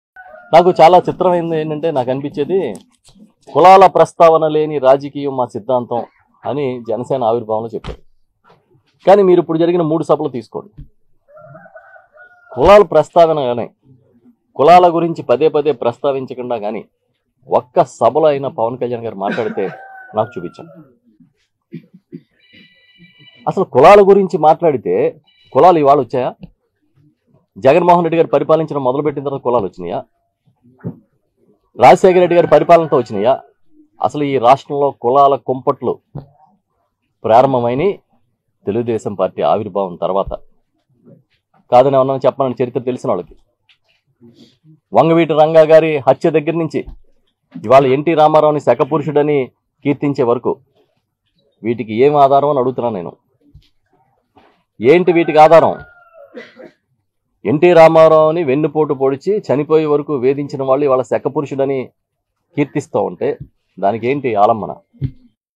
కులం ప్ర‌స్తావ‌న లేకుండా ఒక్క స‌భ‌లోనైనా మాట్లాడారా?
కులాల ప్ర‌స్తావ‌న లేని రాజ‌కీయం మా సిద్దాంతం అని జ‌న‌సేన ఆవిర్భావం సంద‌ర్భంగా చెప్పిన ప‌వ‌న్‌ క‌ళ్యాణ్‌.. కులం ప్ర‌స్తావ‌న లేకుండా ఒక్క స‌భ‌లోనైనా మాట్లాడారా? తెలుగుదేశం పార్టీ ఆవిర్భావం త‌ర్వాతే రాష్ట్రంలో కులాల కుంప‌ట్లు మొద‌ల‌య్యాయి.

- క‌న్న‌బాబు, మాజీ మంత్రి